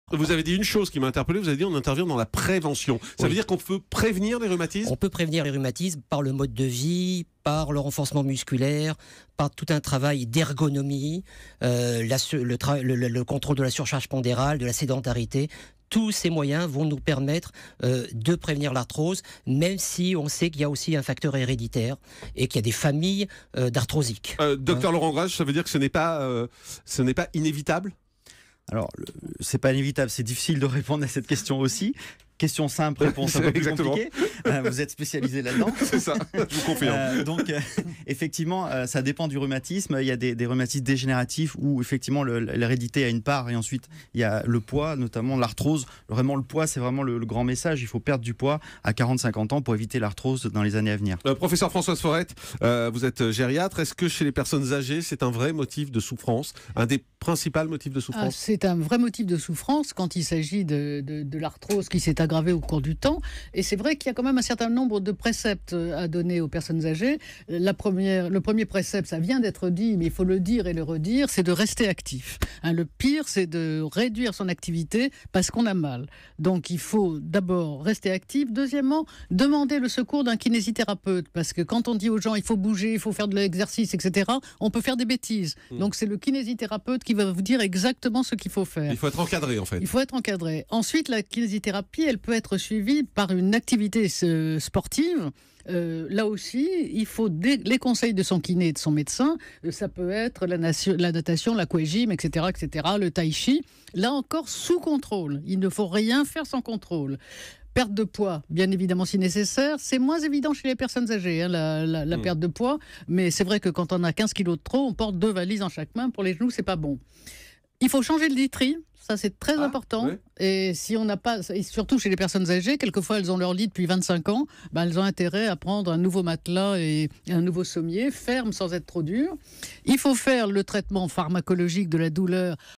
Comment prévenir les rhumatismes ?
Les spécialistes du Grand direct de la santé ont fait le point sur les différents types de rhumatismes et les manières de les prévenir.